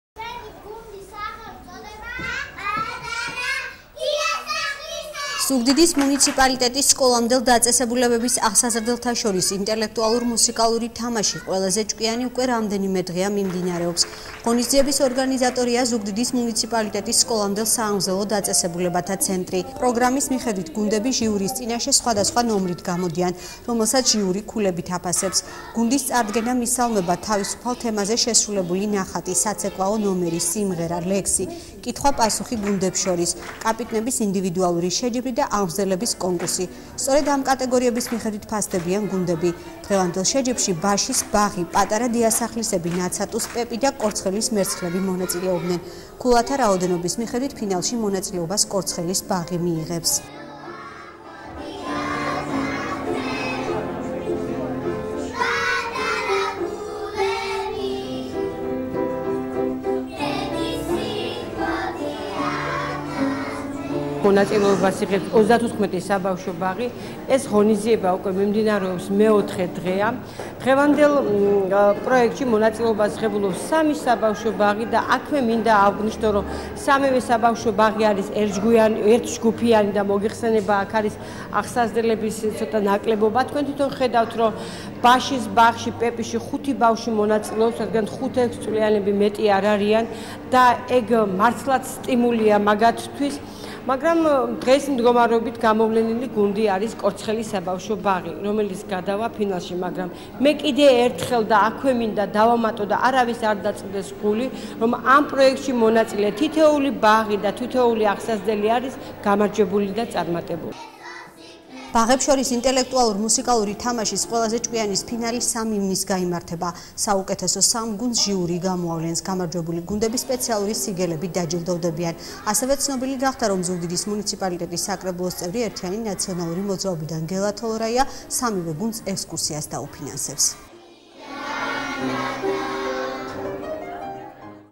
საუკეთესო 3 გუნდს ჟიური გამოავლენს.
ზუგდიდის მუნიციპალიტეტის სკოლამდელ დაწესებულებების აღსაზრდელთა  შორის ინტელექტუალურ- მუსიკალური თამაში „ყველაზე ჭკვიანი“ უკვე რამდენიმე დღეა მიმდინარეობს.ღონისძიების ორგანიზატორია ზუგდიდის მუნიციპალიტეტის სკოლამდელ სააღმზრდელო დაწესებულებათა ცენტრი. პროგრამის მიხედვით გუნდები ჟიურის წინაშე სხვადასხვა ნომრით  გამოდიან, რომელსაც ჟიური ქულებით აფასებს. გუნდის წარდგენა- მისალმება, თავისუფალ თემაზე შესრულებული ნახატი, საცეკვაო ნომერი, სიმღერა, ლექსი, კითხვა პასუხი გუნდებს შორის შორის, კაპიტნების ინდივიდუალური შეჯიბრი და აღმზრდელების კომნკურსი- სწორედ ამ კატეგორიების მიხედვით ფასდებიან გუნდები. დღევანდელ შეჯიბრში ბაშის ბაღი „პატარა დიასახლისები“, ნაცატუს „პეპი“ და კორცხელის „მერცხლები“ მონაწილეობდნენ. ქულათა რაოდენობის მიხედვით ფინალში მონაწილეობას კორცხელის ბაღი მიიღებს.ბაღებს შორის ინტელექტუალურ - მუსიკალური თამაშის „ყველაზე ჭკვიანის“ ფინალი 3 ივნისს გაიმართება. საუკეთესო 3 გუნდს ჟიური გამოავლენს. გამარჯვებული გუნდები სპეციალური სიგელებით დაჯილდოვდებიან.ასევე ცნობილი გახდა რომ ზუგდიდის მუნიციპალიტეტის საკრებულოს წევრი ერთიანი ნაციონალური მოძრაობიდან გელა თოლორაია სამივე გუნდს ექსკურსიას დაუფინანსებს.